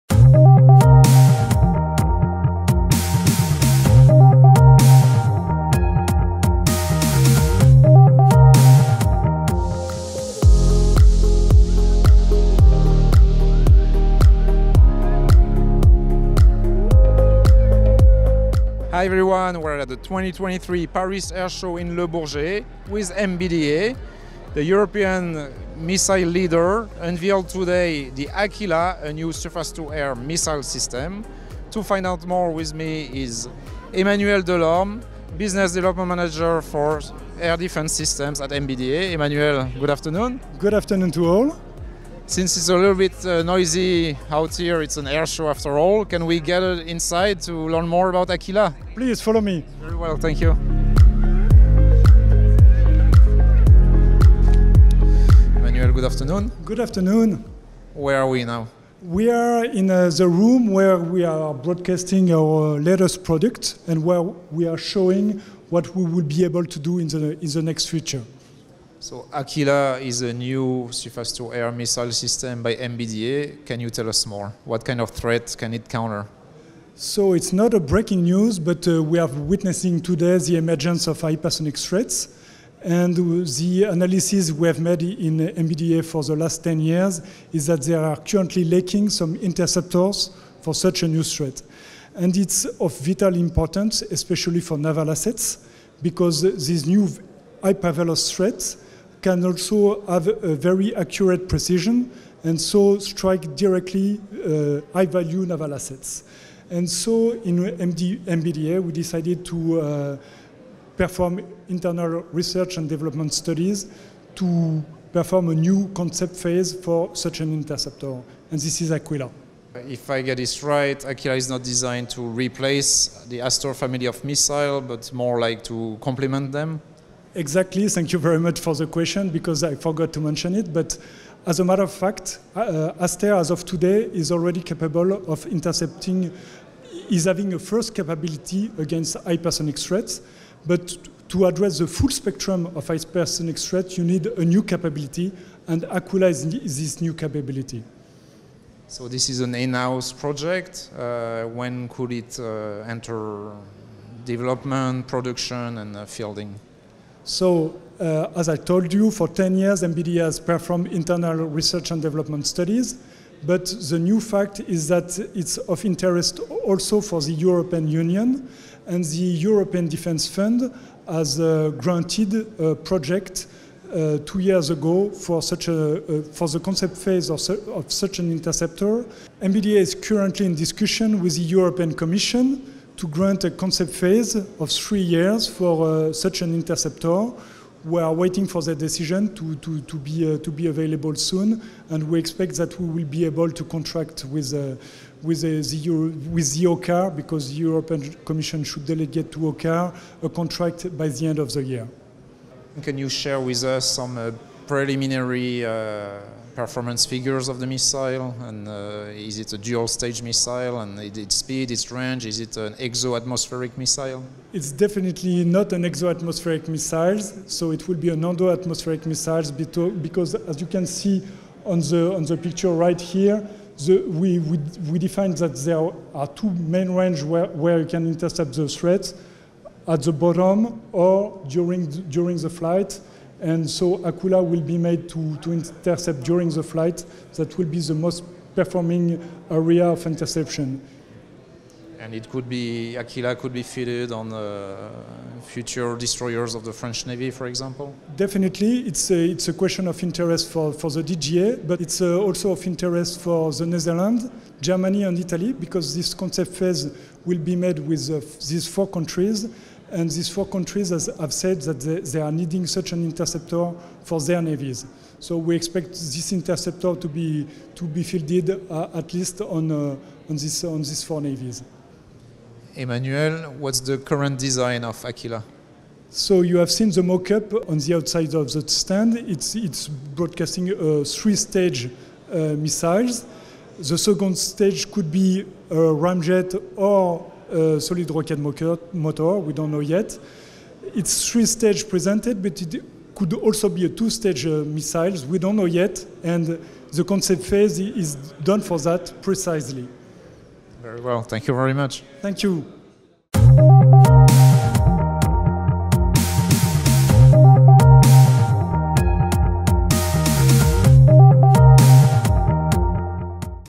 AQUILA: MBDA's new Hypersonic Threats Interceptor
At the Paris Air Show 2023, MBDA unveiled AQUILA: A new land and ship-based interceptor designed to counter maneuvering ballistic missiles, hypersonic cruise missiles and hypersonic glide vehicles (HGV).

MBDA has worked for more than five years on innovative options for counter-hypersonic air-defence building on our expertise from the ASTER anti-missile interceptor and our deep knowledge of threats. This is the AQUILA project, proposing the most valuable counter-hypersonic interceptor concepts for European nations, alongside a global area defence portfolio with other MBDA air defence products.

In March 2023, MBDA was invited with its partners to tender for a concept architecture and technology maturation study of an endo-atmospheric interceptor against the new high end emerging threats.

HYDIS² is the HYpersonic Defence Interceptor Study proposed by MBDA with a consortium bringing together 19 partners and more than 30 subcontractors across 14 European countries

The objective of HYDIS² is to design various interceptor concepts and mature the associated critical technologies to deliver the best interception solution that fulfils the four member states needs - France, Italy, Germany and The Netherlands - and copes with the European TWISTER capability programme. This is a central element of the contribution of European states to the mission of defence of populations and armed forces, in particular against the emerging hypersonic threats that constitute a radical change from ballistic ones.

France, Germany, Italy and the Netherlands have already confirmed their support and commitment by signing a Letter of Intent and agreeing on Initial Common Requirements.